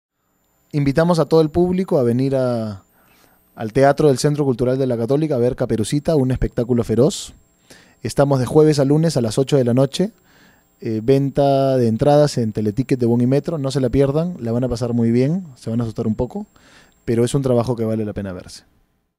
Festival Otras A-puestas: Caperucita (un espectáculo feroz) - Adrián Galarcep Vidal 6
De Javier Daulte
Dirección Adrián Galarcep Vidal
"Tres generaciones de mujeres, un hombre enloquecido de amor, una comedia sobre el amor y sus excesos."
(Algunos cuentos no son para niños)